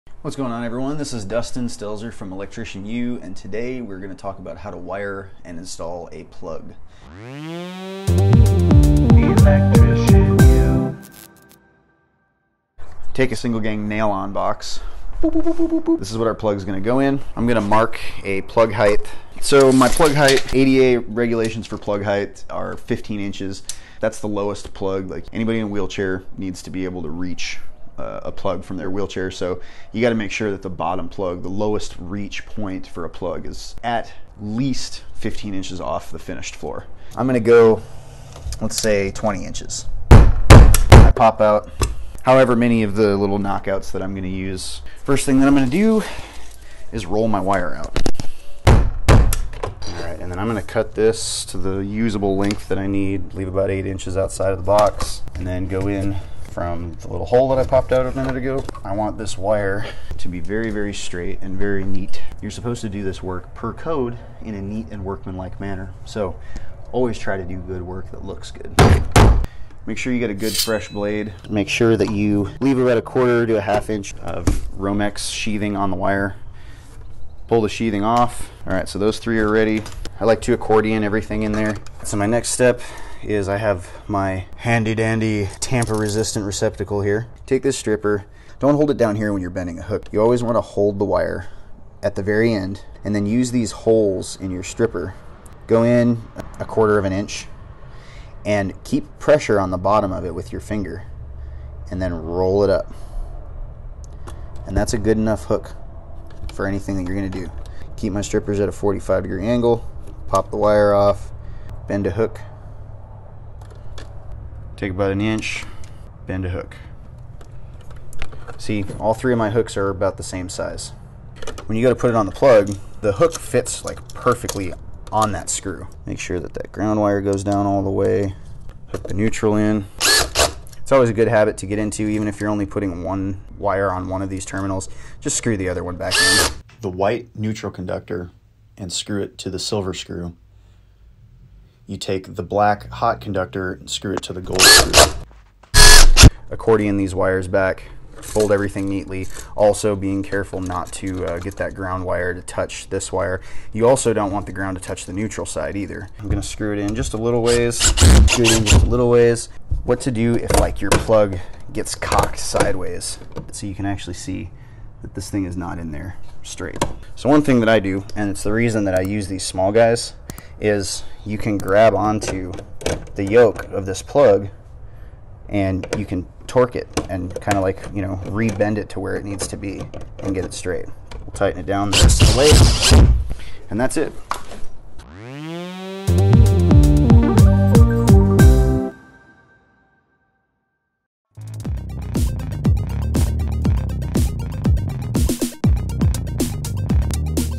SHORTS - How to install a receptacle (in 3 minutes) - SHORT VERSION
This is the short and sweet version of the video I did on this many years ago.  Many have asked to give more bite-sized, concise versions of my videos so here you go!  For the full version of this video

00:00​ Intro
00:12 1st Step - Single Gang Nail On Box​/ Plug Height
00:42​ Pop Out Knock Outs & Roll Wire Out
01:02 Straight & Neat - PER CODE​
01:33 Hook Wire​ & Attach To Plug
02:27 Accordion Wires In, & Screw Plug in... NEATLY ​
03:05​ Outro

-Audio lessons
-Forum

-State Approved
-Video Based

-2017, 2020, and 2023 NEC versions
-300 Question Online Code Cannon (not license specific, all code)

Plugs are pretty day one helper stuff, but I talk about some methods of twisting joints, using strippers, cutting sheathing, and mounting the devices that maybe some teachers out there don't use.

As an apprentice, getting used to how your tools work and practicing with them over and over will get you better faster.  It will take a lot of running romex, stripping the ends out, and twisting hooks before this comes naturally.  But you WILL get there quickly.

In this show I take 12/2 romex from an existing switch box that has power in it already.  From there I install a plug box below and staple the wire in between the two boxes.  Finally I make joints in the switch box to feed the plug hot, and install the receptacle in the box below.

After everything is done it is important that you ALWAYS CHECK YOUR WORK.  Always put a tester in of some sort and make sure every electrical installation you touch works before you walk away from it.  Not just for plugs and switches, but for EVERYTHING you do.  I use a simple plug tester at the end to ensure everything is wired correctly and we have electrical power present at the receptacle.